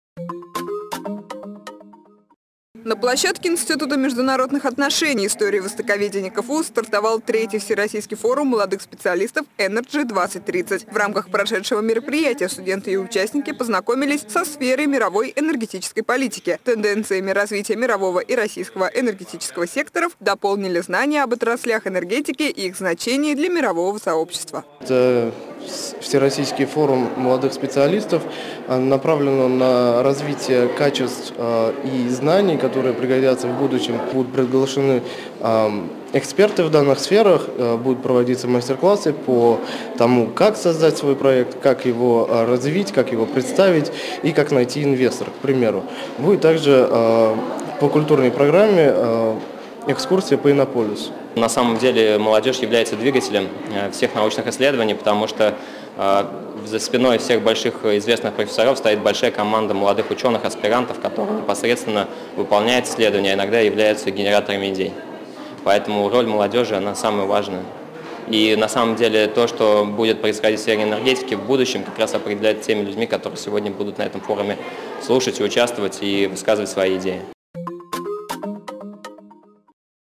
Energy 2030
Студенты Казанского университета приняли участие в форуме молодых специалистов Energy 2030. Подробности о состоявшемся мероприятии расскажет Аделя Шемелова